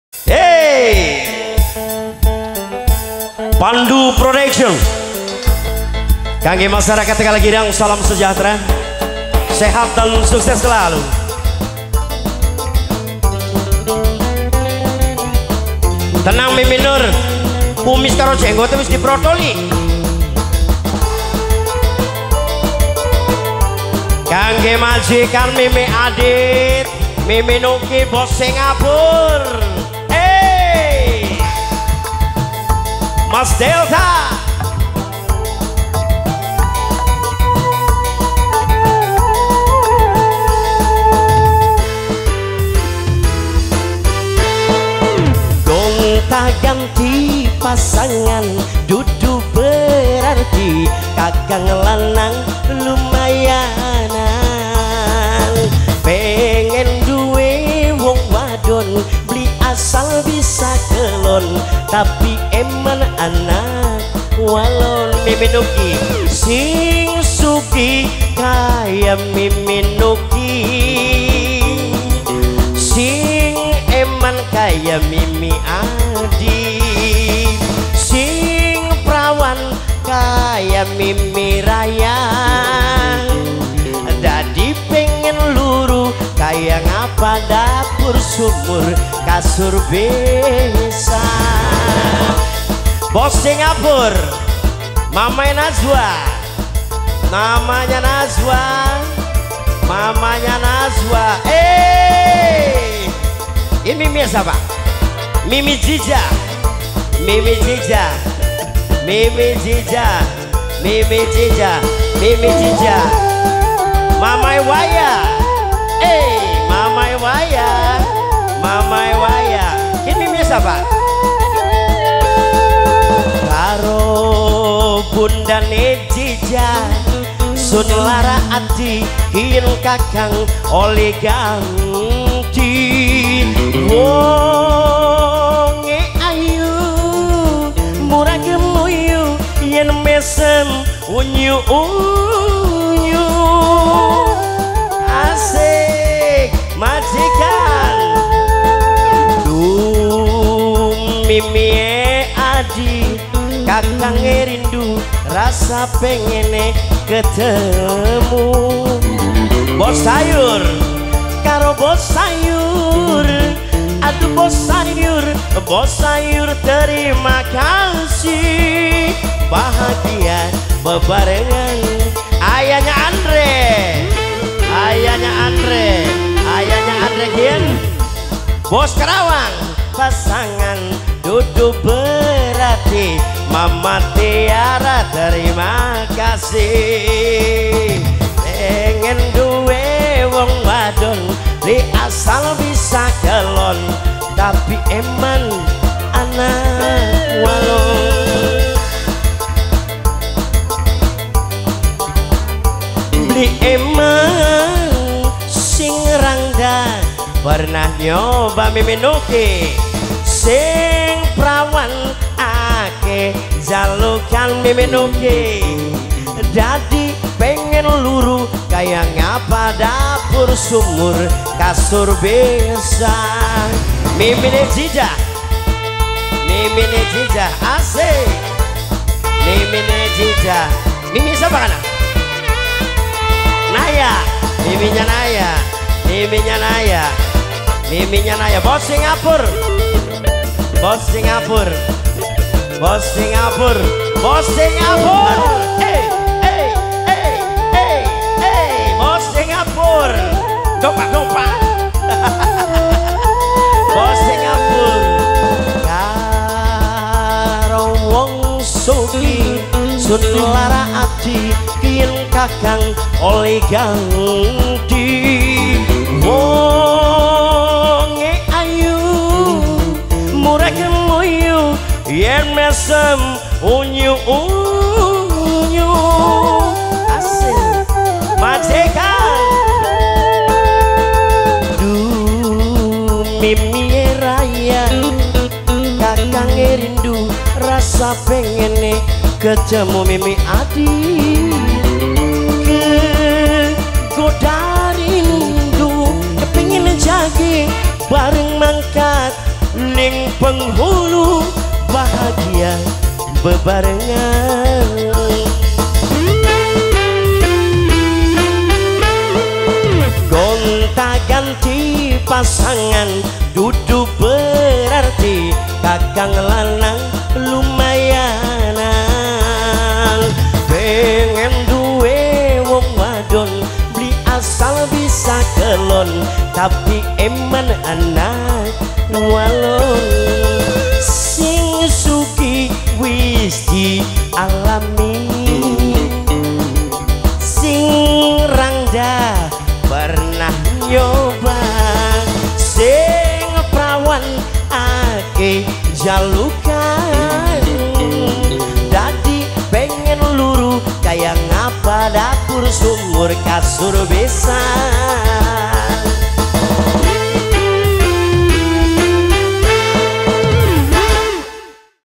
UNYU UNYU BRS ENTERTAINMENT TEGALGIRANG
DOKUMENTASI KEL. BPK. MADI / IBU CARMINIH DALAM RANGKA KHITANAN  ADITYA PRATAMA YANG DI LAKSANAKAN PADA HARI SENIN 17 MEI 2021 BERTEMPAT DI DESA TEGALGIRANG BLOK TEGALGIRANG
KEC. BANGODUA - INDRAMAYU